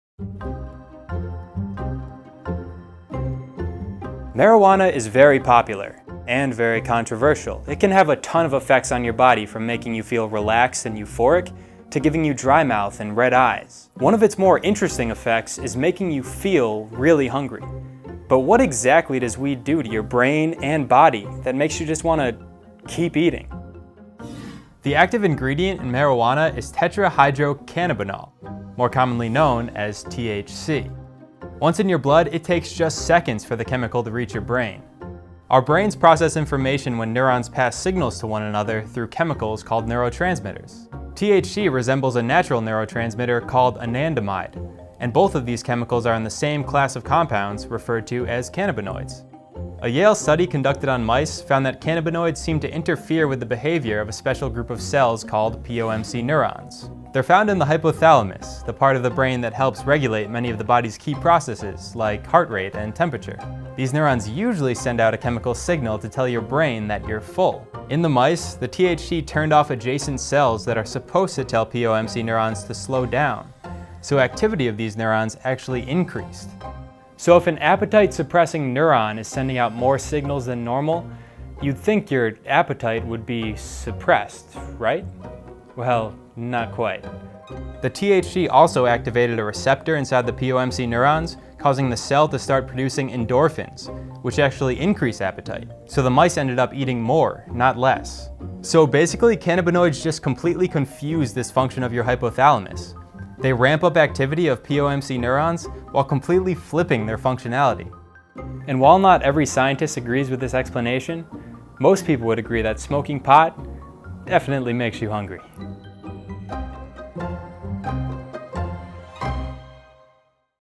Why Marijuana Gives You The Munchies
Marijuana can have various effects on your body. But what is it about weed that makes you so hungry?